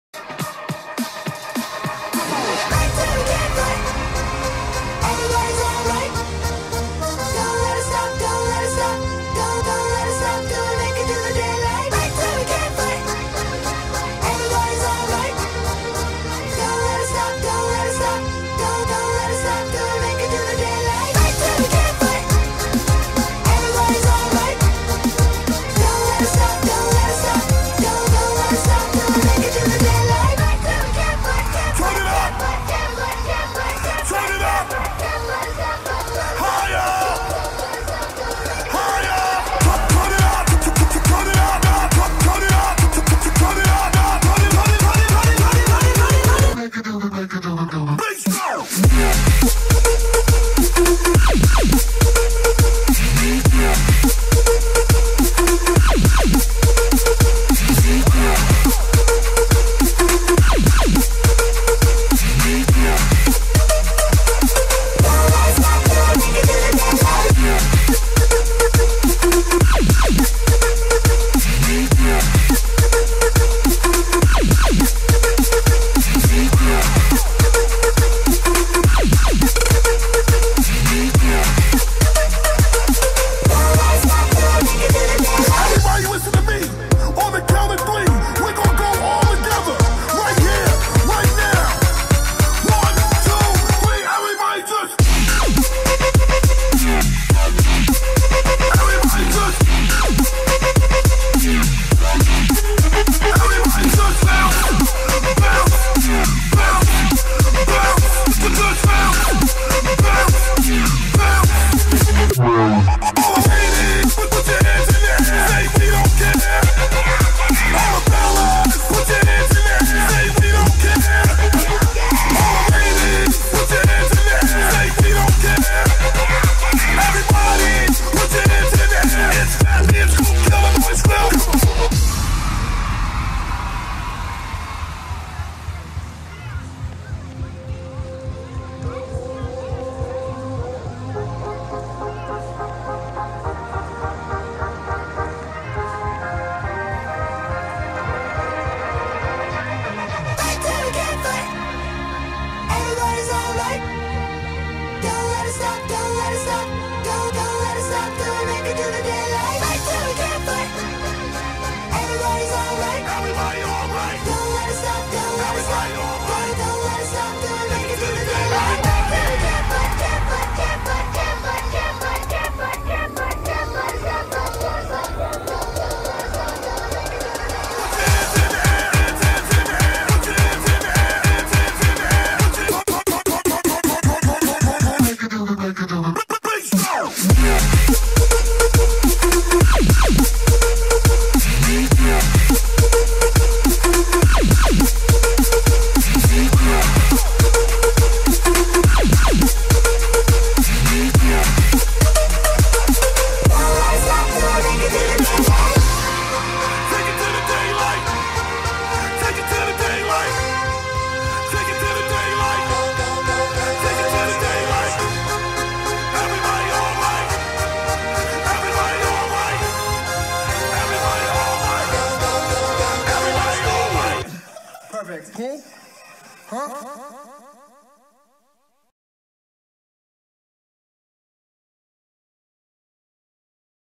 EMOB | Victoria Falls Zambia, Animation Dance | Mi6 CREW
i danced at the Victoria falls